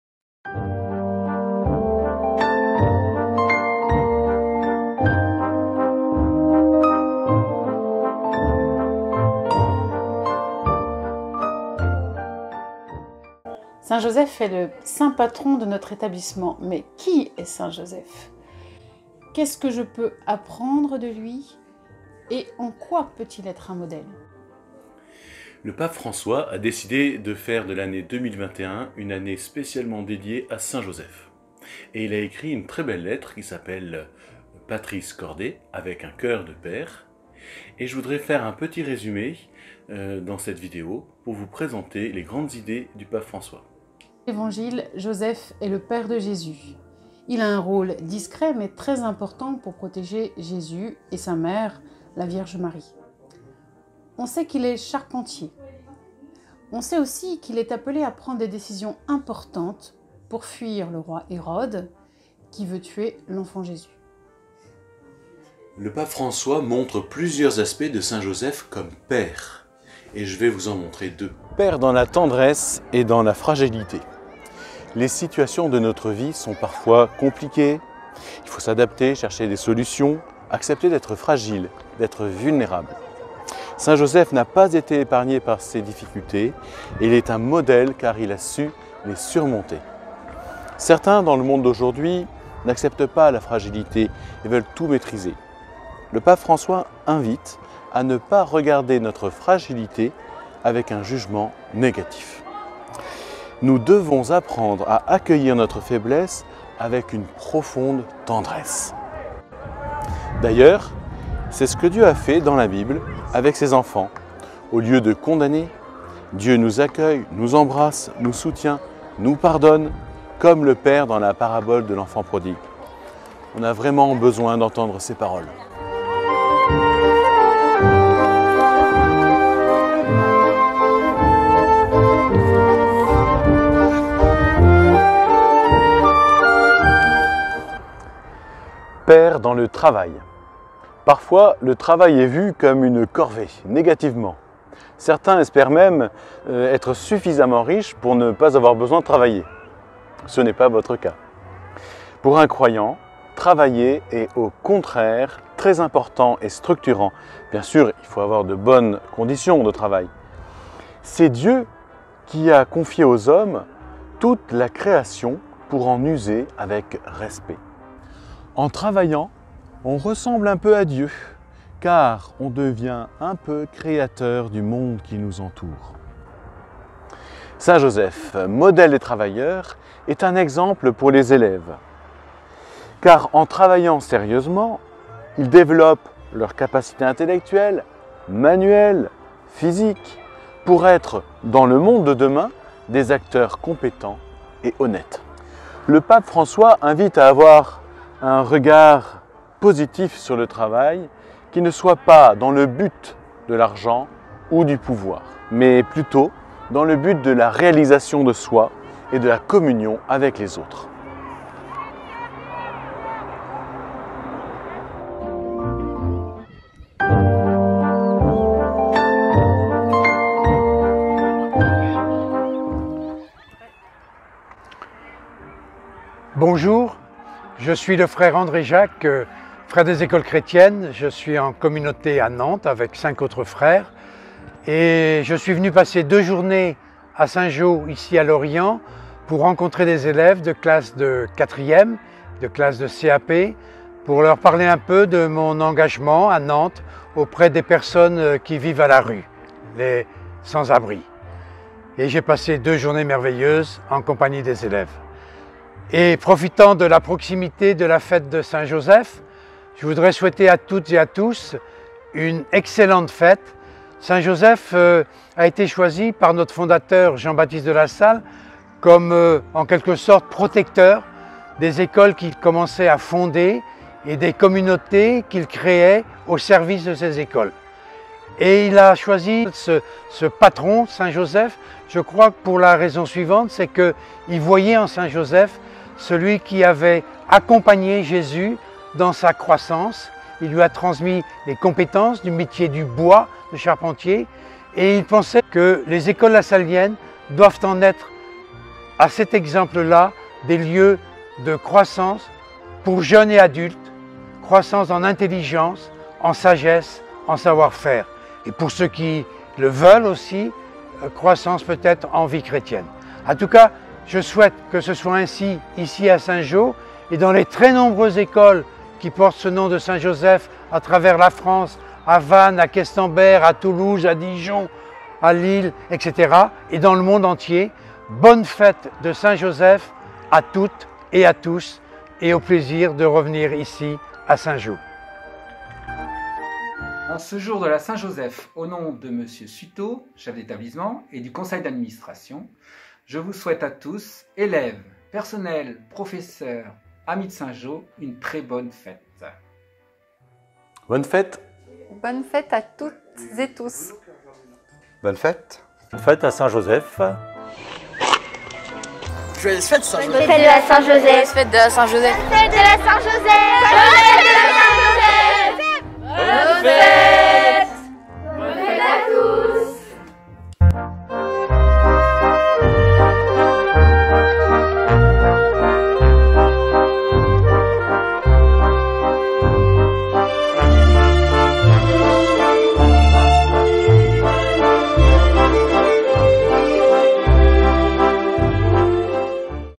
Fête de Saint Joseph 2021 à Saint Joseph-La Salle LORIENT
Bonne fête de Saint Joseph !
En cette année sous le patronage de Saint Joseph, Saint Patron et protecteur de l’Institut des Frères des écoles chrétiennes, arrêtons nous quelques instants le 19 mars pour le fêter particulièrement.
La lettre du Pape « Patris Corde » nous rend vivante la personne de ce grand saint sur lequel nous savons si peu de choses !